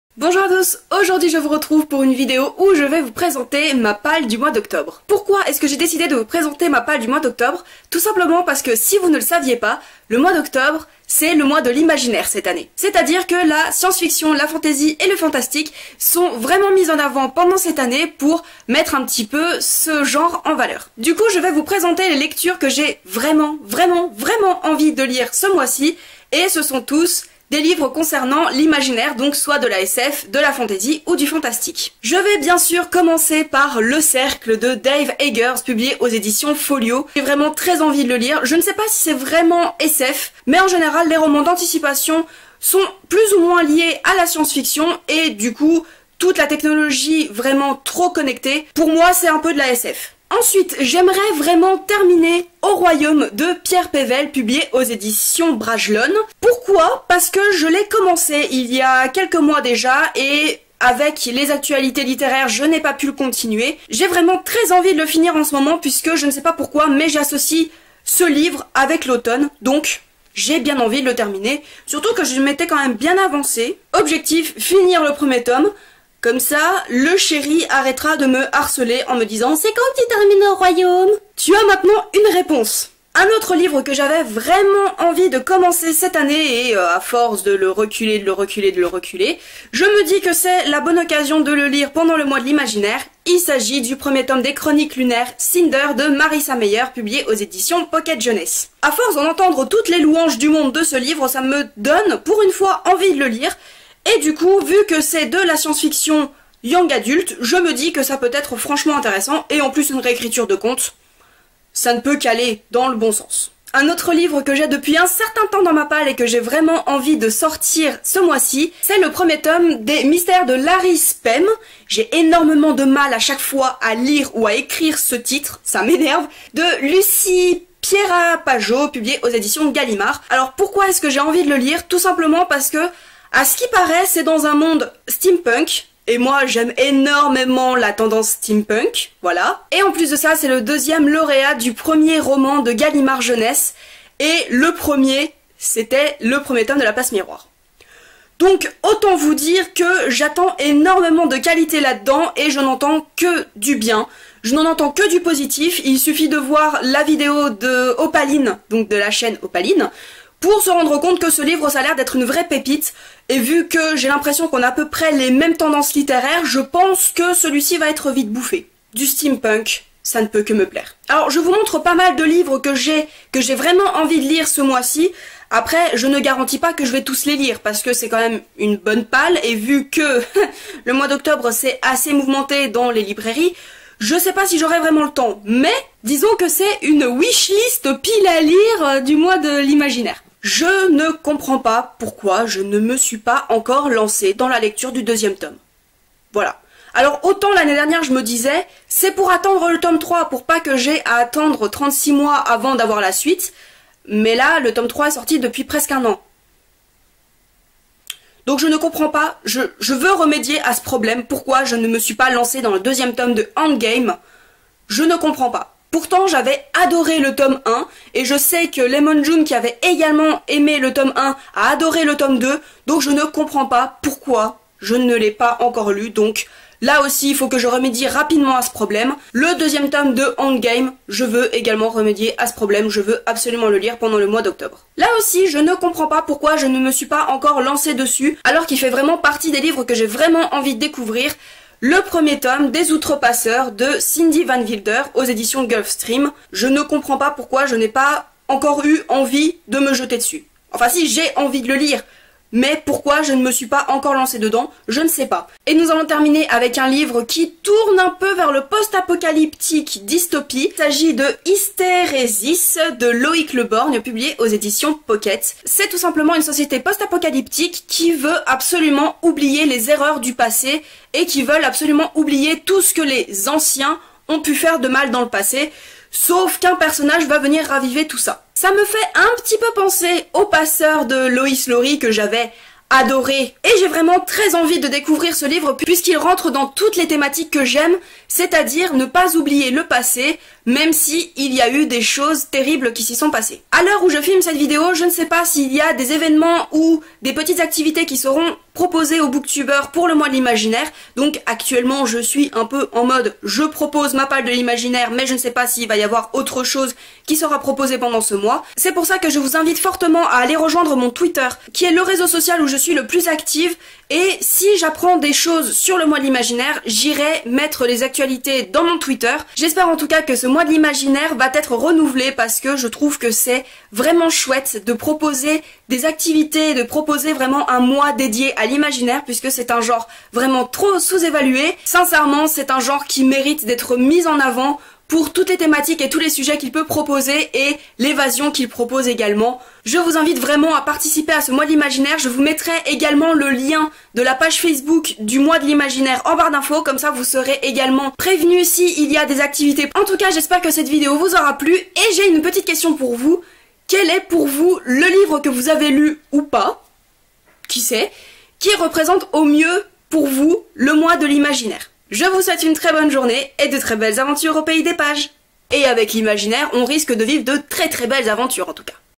PAL | Mois de l'imaginaire
Aujourd'hui je vous retrouve avec ma première vidéo sur le Mois de l'Imaginaire, et j'espère que ça ne sera pas la dernière !

Le Mois de l'Imaginaire est un projet proposé lors des Imaginales, et c'est pour ce mois d'octobre ! Et j'espère que ça ne sera pas la seule fois que ce mois sera mis en avant !

En attendant de voir s'il y aura d'autres vidéos de proposée, je vous présente la PAL que je me suis concocté pour ce mois, sous le signe, comme vous l'aurez compris, de l'imaginaire.

Voici la liste des livres cités :
- Le cercle, de Dave Eggers (Folio);
- Haut-Royaume, de Pierre Pevel (Bragelonne);
- Cinder, Marissa Meyer (Pocket Jeunesse);
- Les Mystères de Larispem, Lucie Piera-Pajot (Gallimard Jeunesse);
- Endgame, de James Frey et Nils Johnson-Shelton (Gallimard Jeunesse);
- Les Outrepasseurs, de Cindy Van Wilder (GulfStream);
- Hysteresis, de Loïc Le Borgne (Pocket).

Croisons les doigts pour que je puisse lire tout ça !

Je vous laisse pour l'instant, et on se retrouve bientôt pour une prochaine vidéo !

Je filme avec : un appareil photo Nikon CoolPix S5200.
Je monte avec : Windows Movie Maker.
J'utilise les polices de DAFONT pour mes miniatures.